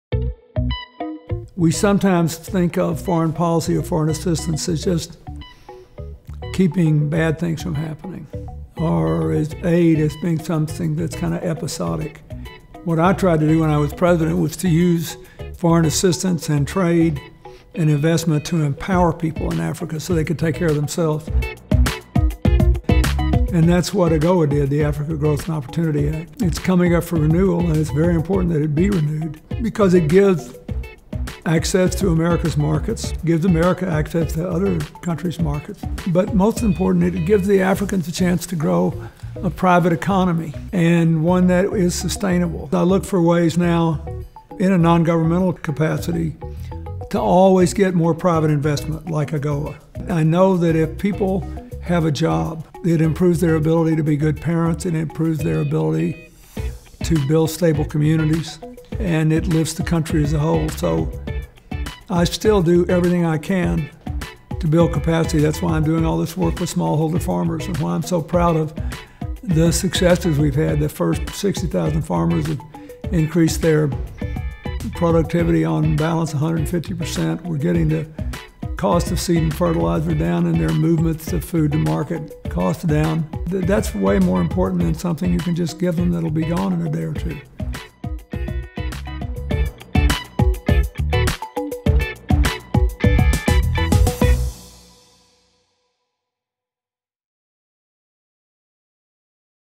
President Clinton Explains the African Growth and Opportunity Act
Fifteen years ago, President Clinton signed the African Growth and Opportunity Act, a bipartisan bill that increased investment in Africa. But his commitment to Africa didn’t end when he returned to private life. The Clinton Foundation has helped over 85,000 farmers in Malawi, Rwanda, and Tanzania increase their incomes through access to improved seeds and training in agronomic techniques.